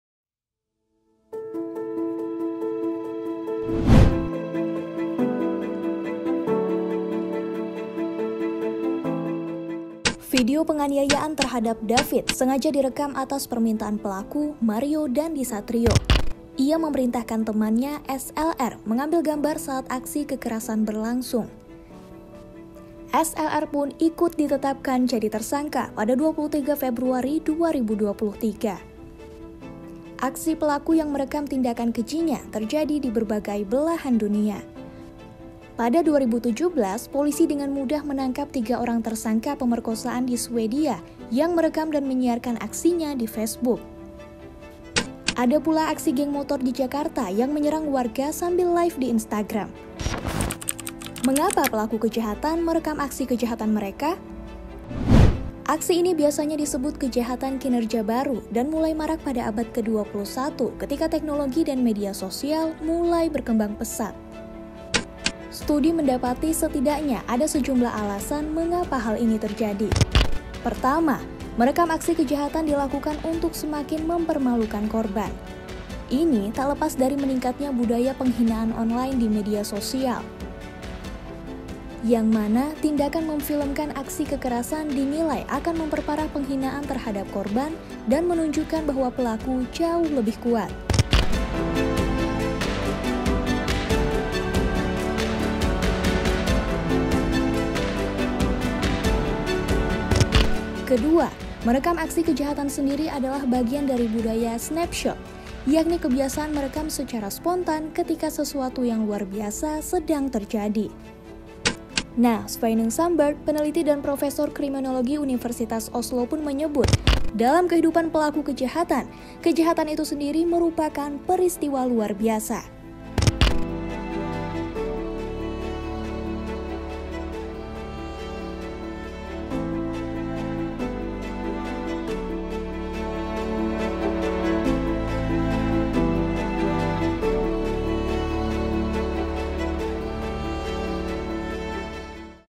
Saat Aniaya David, Mario Minta Aksinya Direkam: Kenapa Pelaku Merekam Aksi Kejahatan? | Narasi Daily
Ini bukan kasus pertama pelaku merekam aksi kejahatan yang mereka lakukan—yang justru bikin mereka makin mudah ditangkap. Apa yang bikin pelaku merekam aksi kejahatan mereka?

(Narasi)